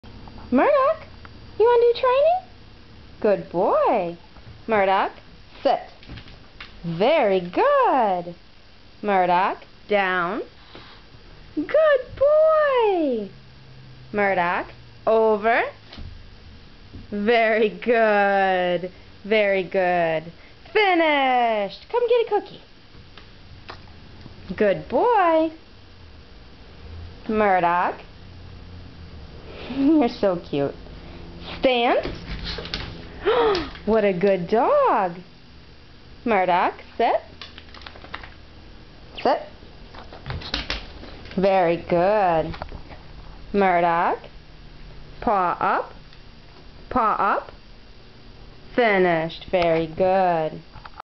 Training the Murd 1
Murdoch does a few commands